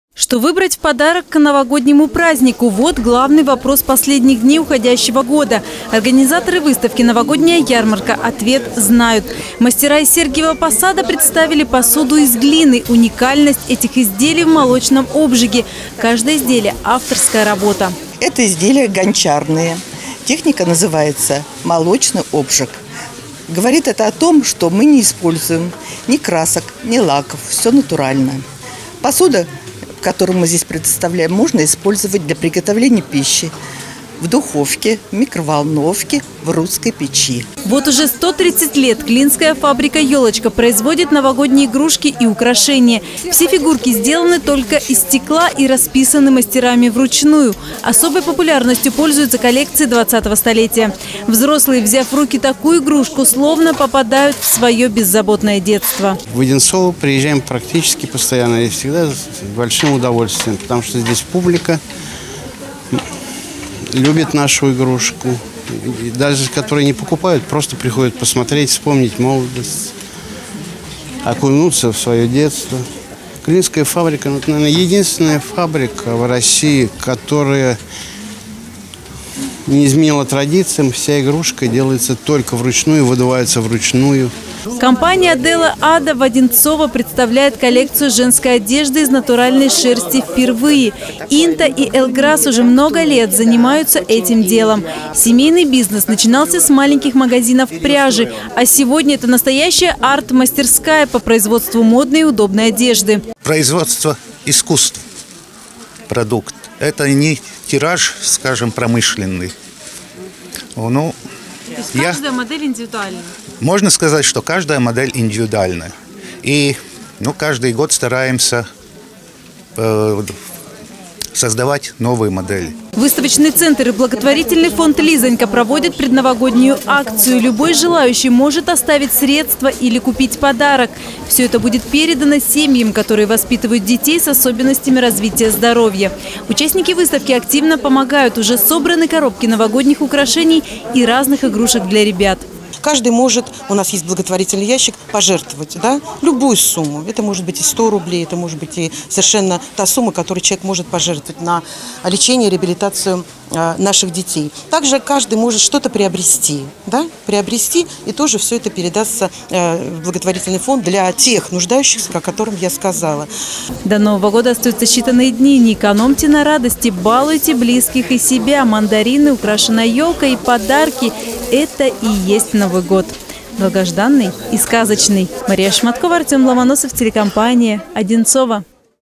Закрытие выставочного сезона – 2017
Последняя ярмарка уходящего календарного года посвящена самому любимому празднику в нашей стране. Здесь вы найдете все от идеи новогоднего подарка, до деликатесов к новогоднему столу. Отечественные производители представили свои лучшие товары. Так что же будут дарить Одинцовы друг другу в новогоднюю ночь – узнала Мария Шматкова.